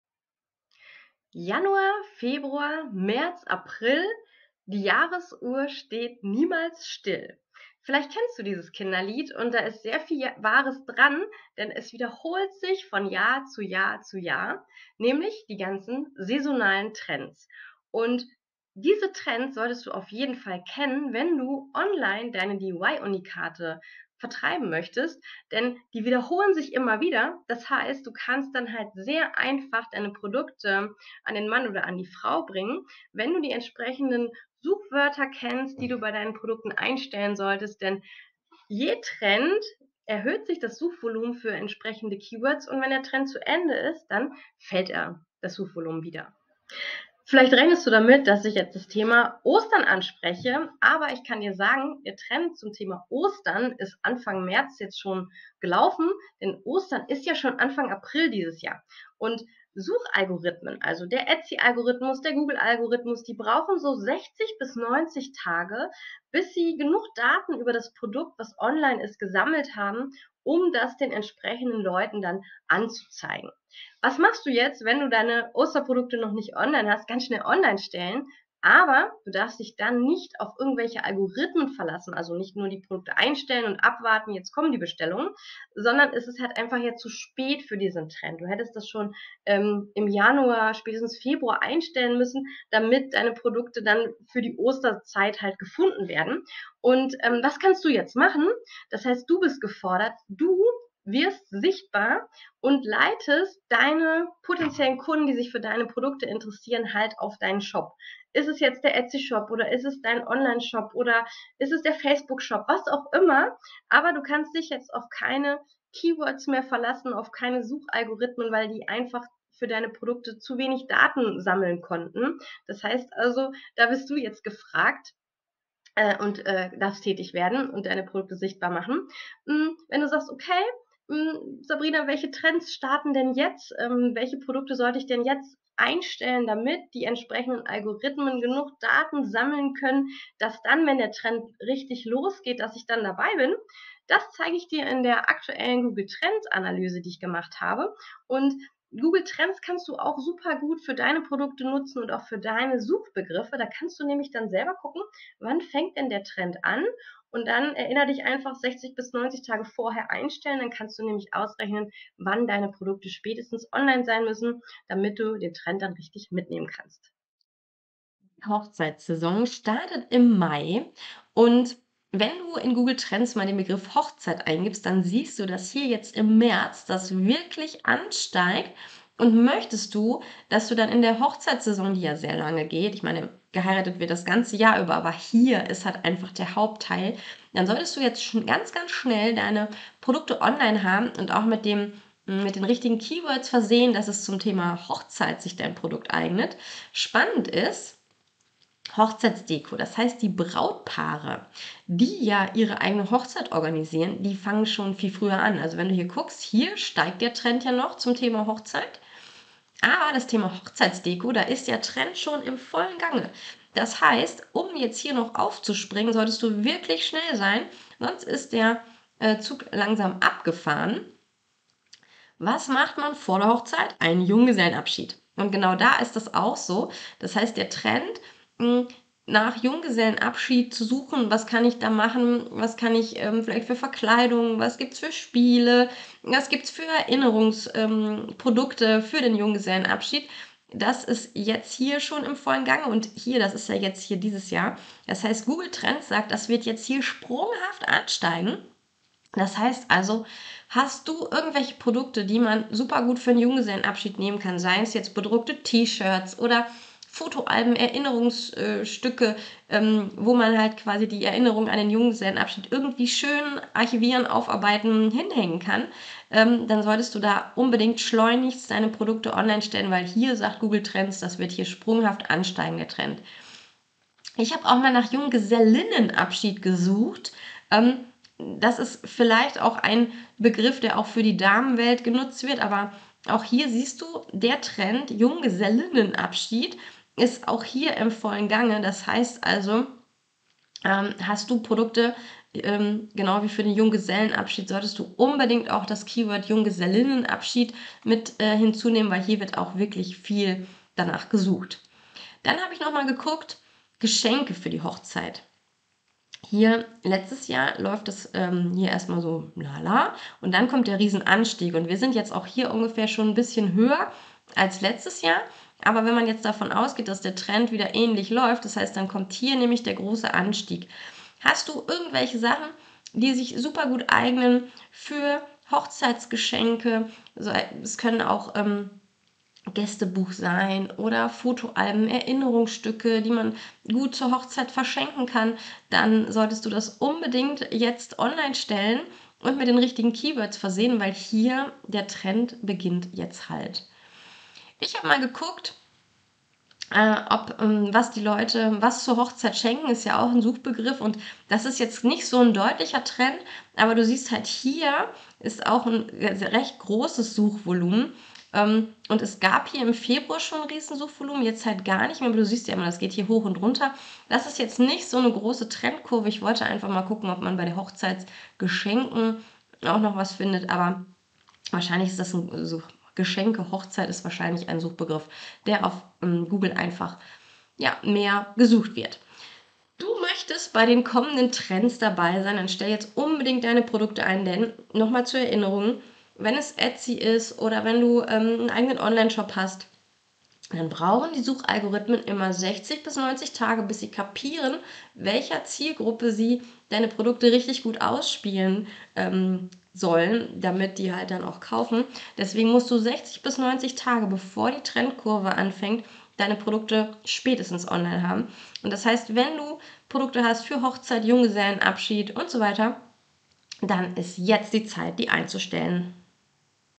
Handmade Trends 2023 - angesagt im März
Januar, Februar, März, April.

Rund ums Jahr gibt es viele saisonale Trends, die kurzfristig oder längerfristig jedes Jahr immer wieder aufs Neue auftreten. Entscheidend ist es für deine DIY Handmade Selbstständigkeit, dass du gleich am Anfang bei dem Trend mit deinen handgefertigten Unikaten dabei bist. Das garantiert dir viele Verkäufe in diesem Trendverlauf.

Damit du deine saisonalen Handmade Produkte gut planen kannst, schaue ich im März bereits auf die Themen, Feste und Anlässe, die im Mai und Juni 2023 stattfinden.

Du möchtest mehr über Trends erfahren und deine DIY Handmade Unikate verkaufen, dann komm zu uns in die DIY Handmade Business Mastermind

Kreative Grüße

Deine Sabrina